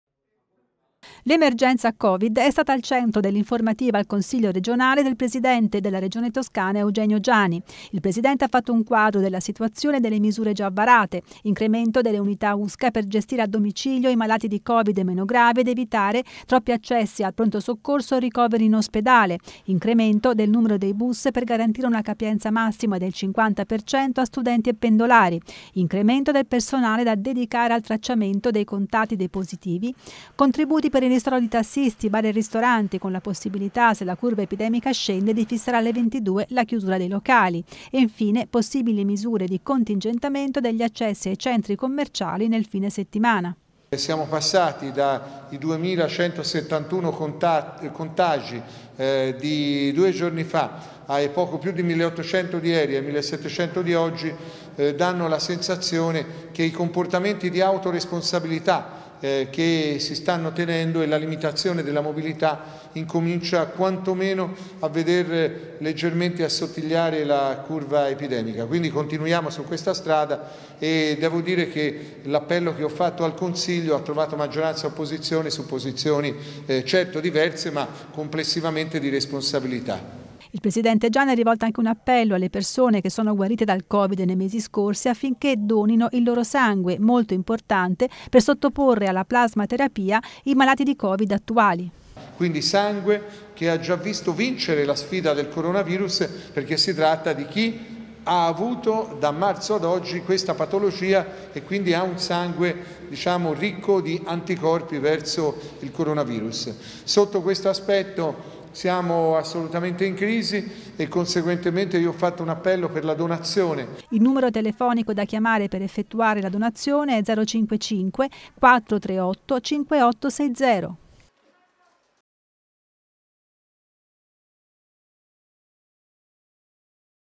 Covid, in Consiglio punto sulla situazione
Nel quadro delle misure già adottate l'aumento delle unità destinate alle cure domiciliari dei malati meno gravi per evitare sovraccarichi ospedalieri, incremento dei numero degli autobus per garantire il rispetto del limite massimo di capienza del cinquanta per cento per ogni mezzo e personale aggiuntivo da dedicare alle attività di tracciamento dei contatti dei positivi. Sul versante delle attività commerciali e di pubblica utilità deliberati contributi a favore di tassisti, gestori di bar e ristoranti e possibili contingentamenti degli ingressi nei centri commerciali durante i fine settimana. La Regione rivolge anche un appello alla donazione del sangue alle persone che sono guarite dal Covid nei mesi scorsi per sottoporre a plasmaterapia i malati attuali. Il numero telefonico da contattare per la donazione è lo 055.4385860.